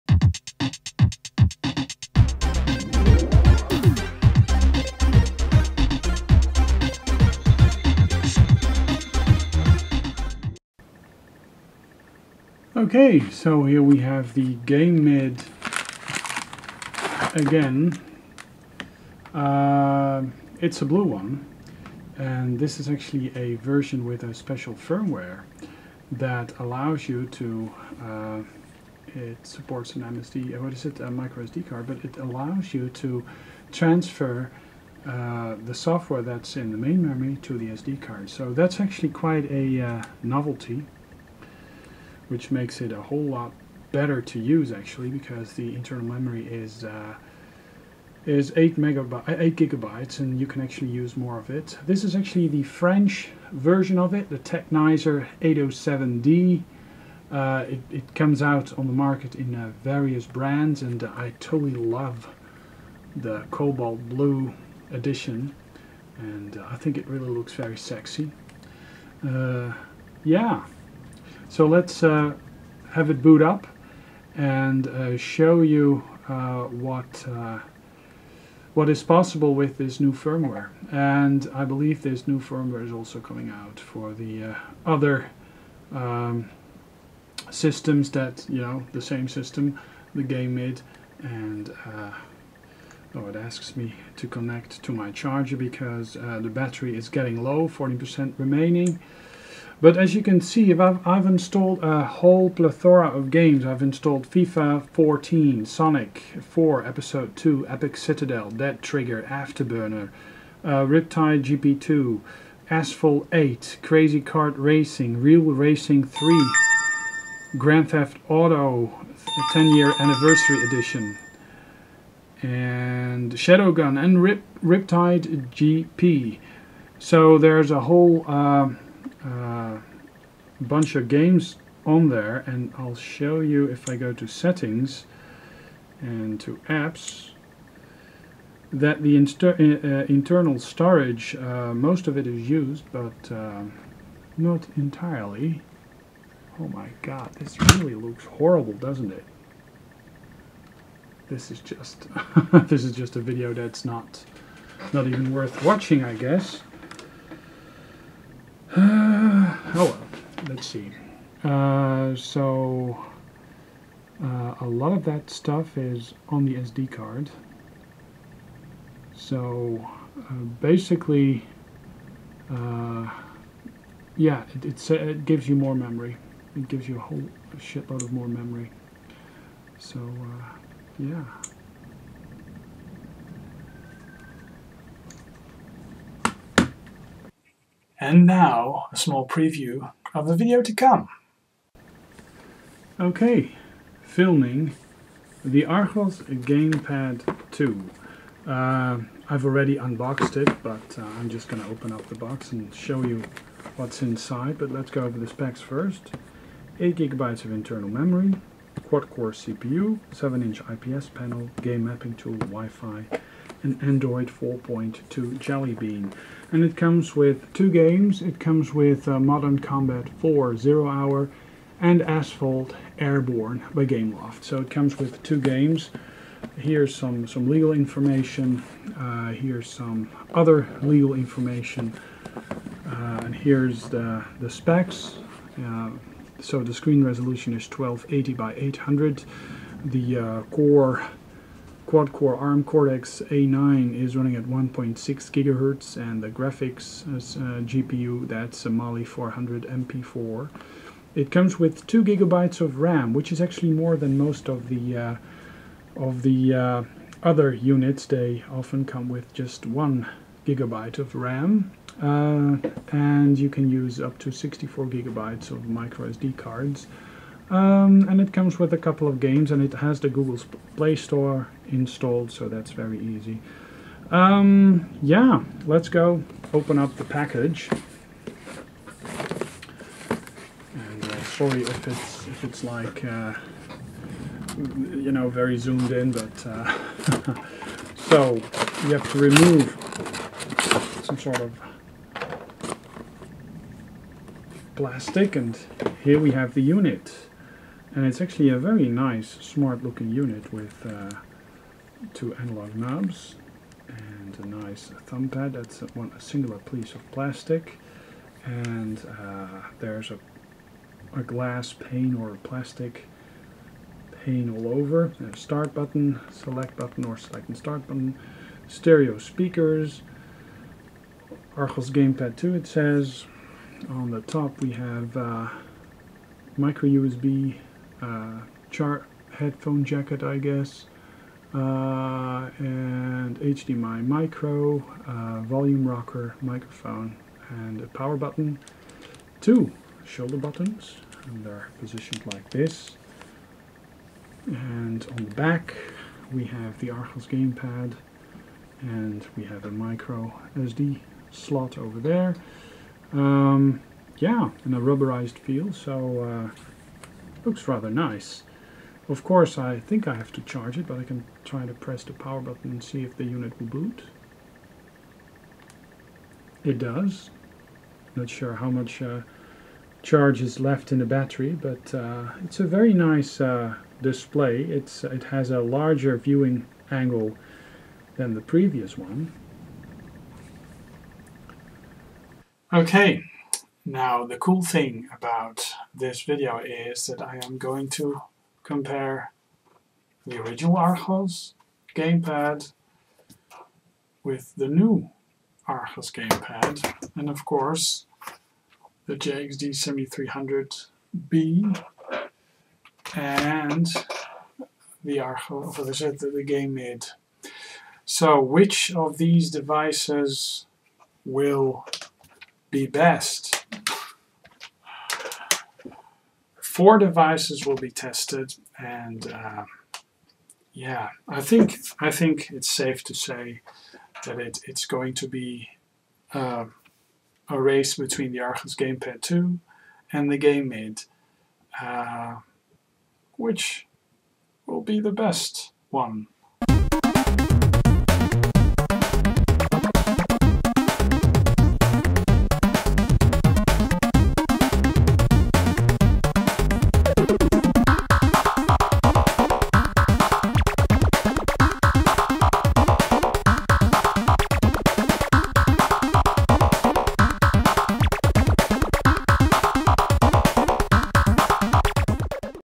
GameMID & Tek807d Update and a Preview of the "Archos Gamepad 2 vs GameMID" video
I got a nice cobalt blue version of the GameMID to test out a new firmware that enables the device to store and install applications onto the SD card as if it was the internal memory. It's best to use class 10 SD cards for it though. And it makes it possible to install quite a few large games on the unit that would not have fitted in the standard 8Gb/Go internal memory the unit comes with. Very nice that a company itself is doing these software improvements so that no third party software is needed.

I also obtained an Archos Gamepad 2 and I want to put it through its paces. I want to see which of the two comes out tops. Hardware wise the Archos Gamepad 2 should be faster but is it?

The video also includes little sample of the Archos Gamepad 2 vs GameMID video I am working on.